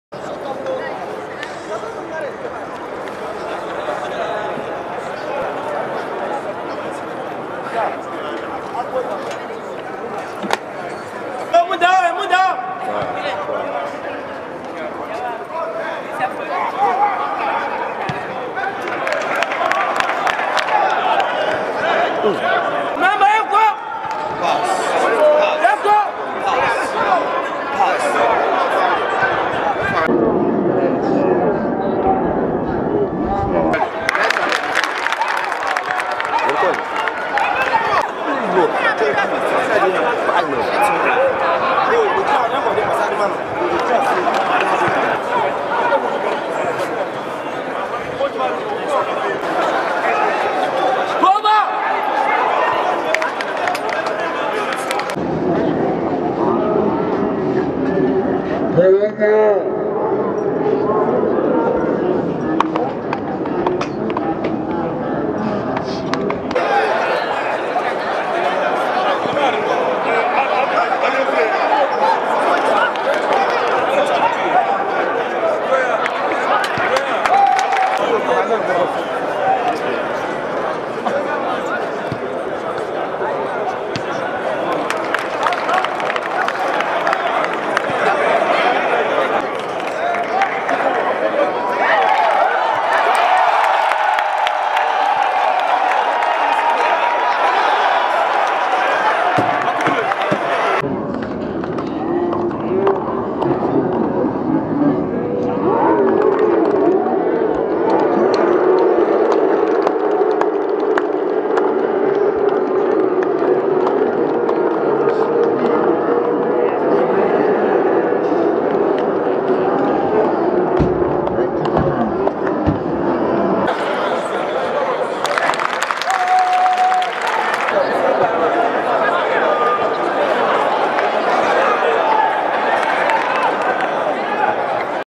Very best of Samuel Bawa(2)
His curently one of the youngest Best Central Defender to watch in Ghana..((very very good Caculative with the and with thout the ball world watch out this player.